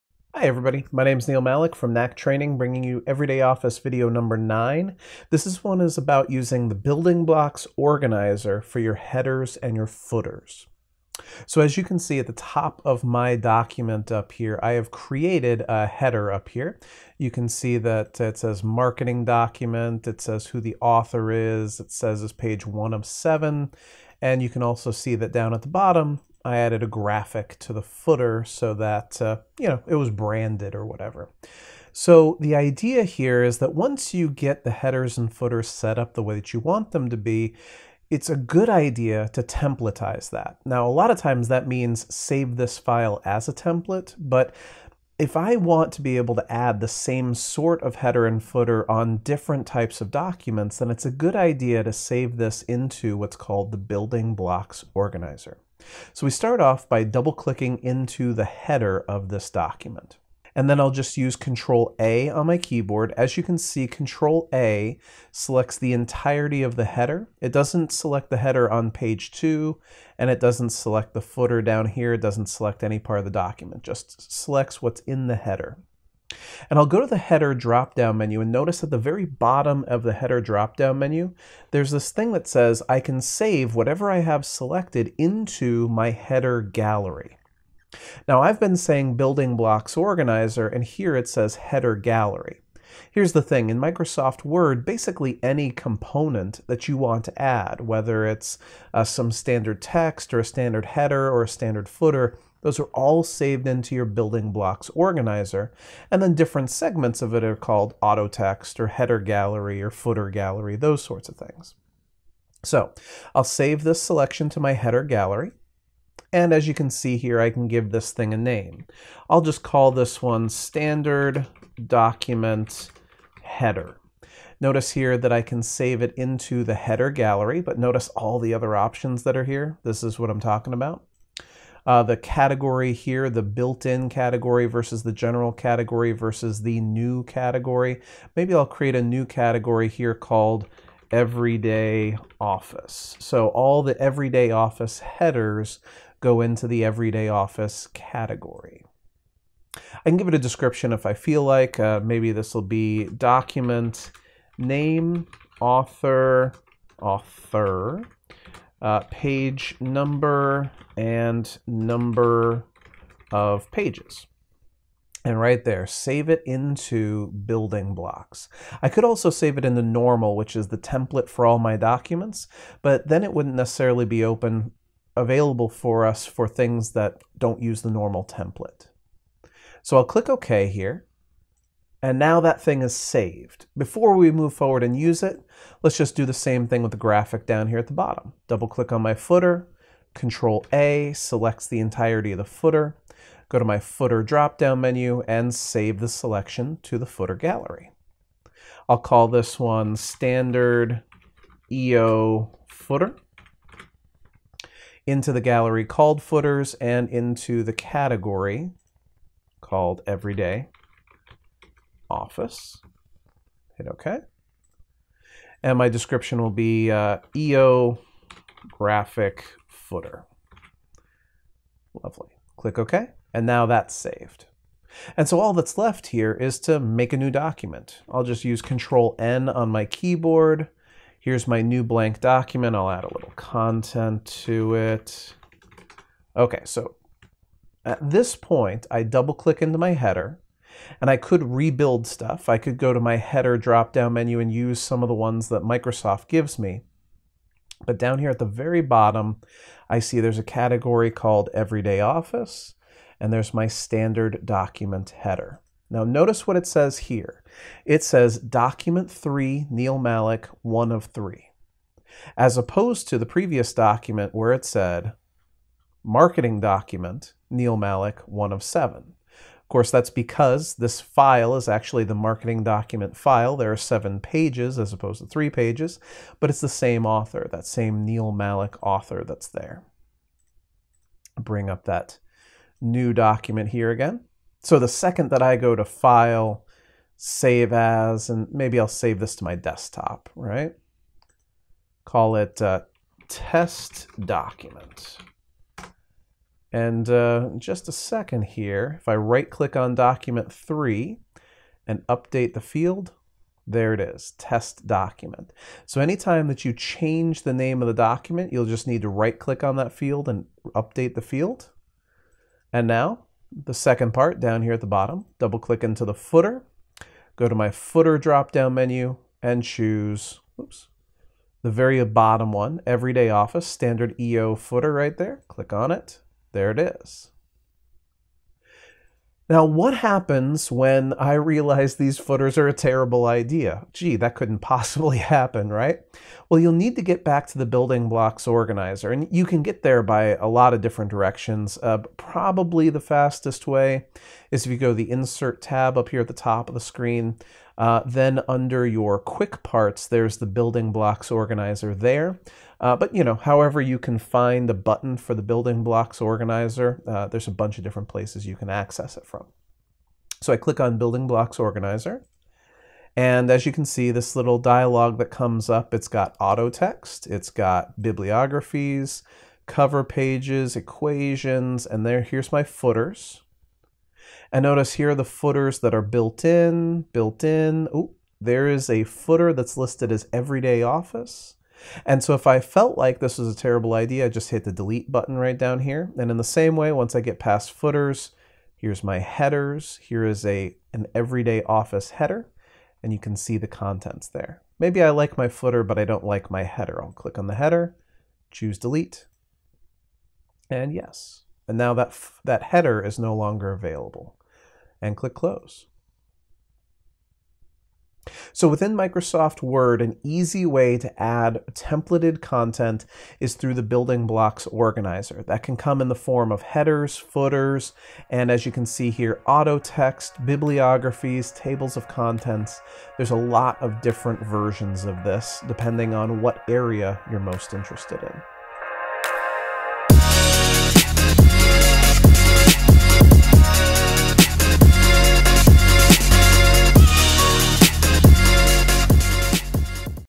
Creating Header and Footer Templates for Word in the Building Blocks Organizer
If you need to reuse your headers and footers from one document on others, save them to your Building Blocks Organizer! This short tutorial demonstrates how to create, save, and reuse your headers and footers.